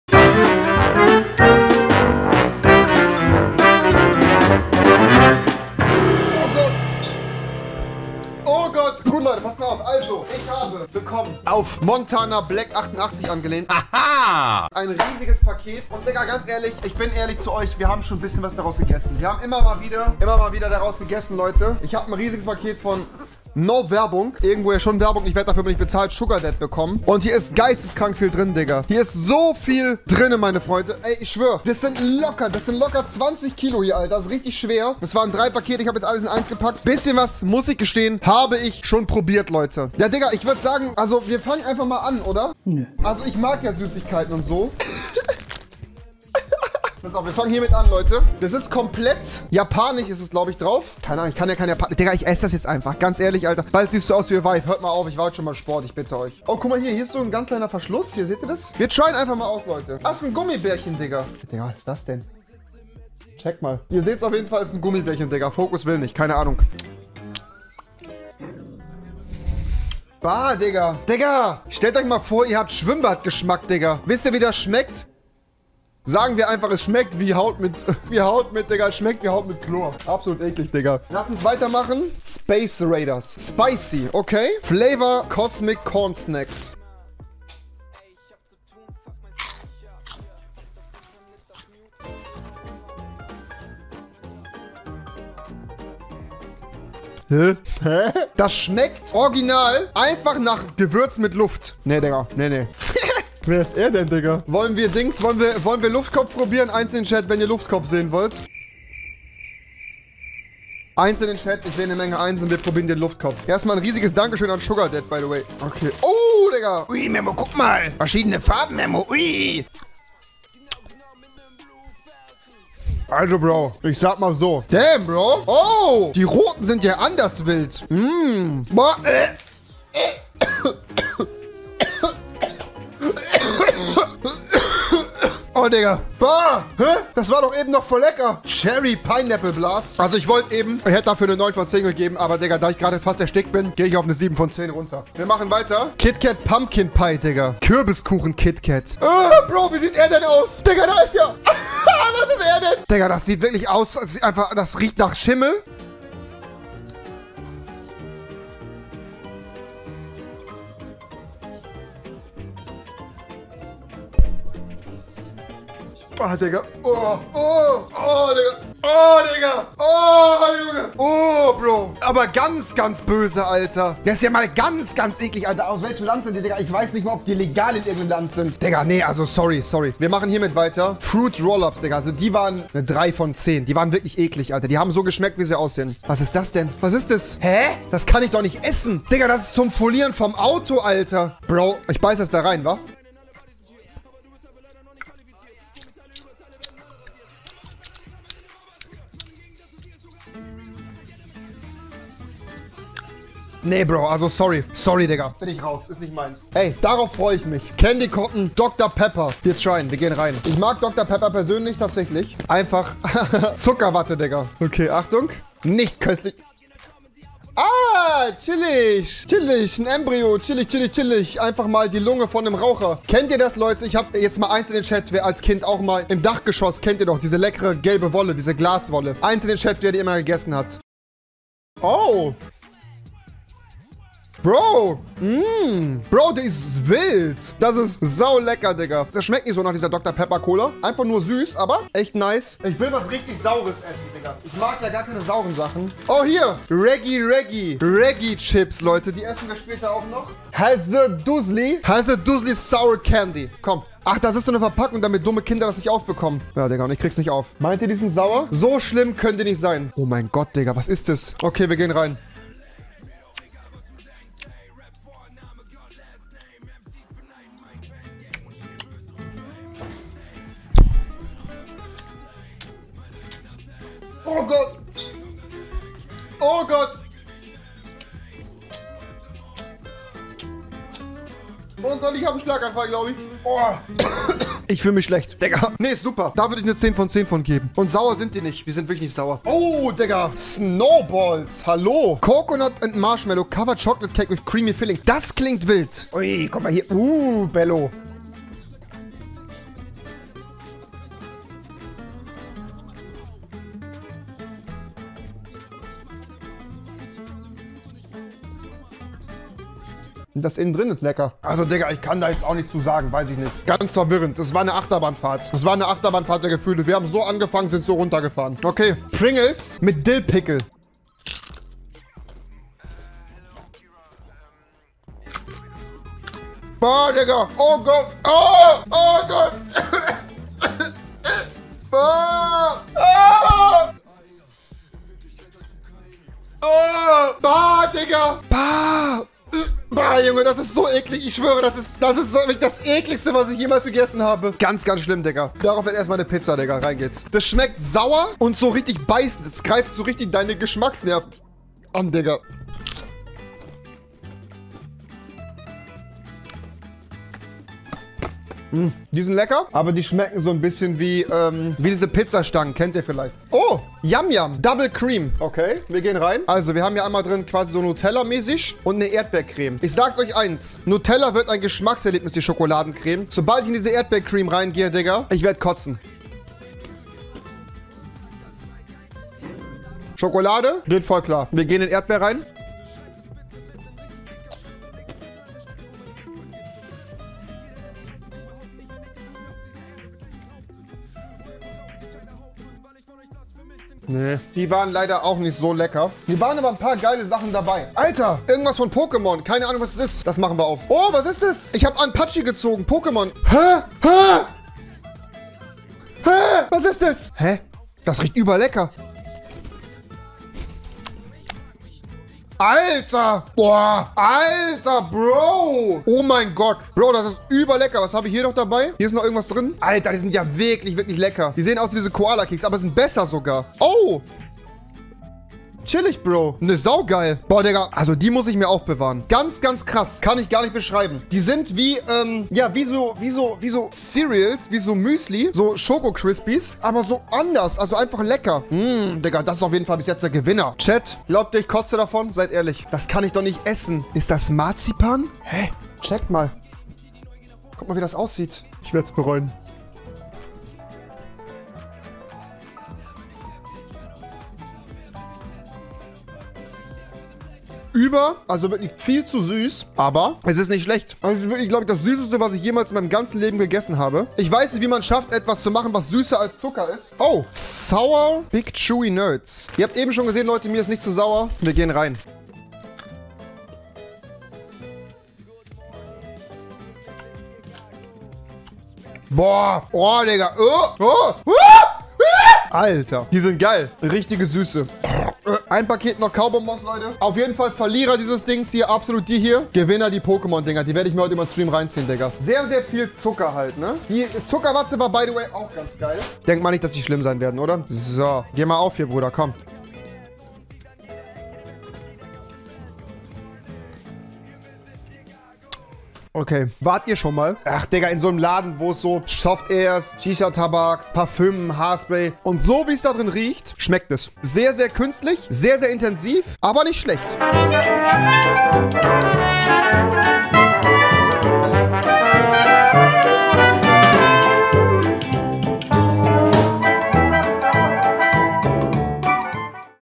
Wegen ekelhaftesten Chips im Stream 🤮... naja AMERICAN CANDY probieren!!
Heute probieren wir mal amerikanische Süßigkeiten! Was für Süßigkeiten mögt ihr am liebsten? 🍭🍬
Vielen Dank an SugarDad!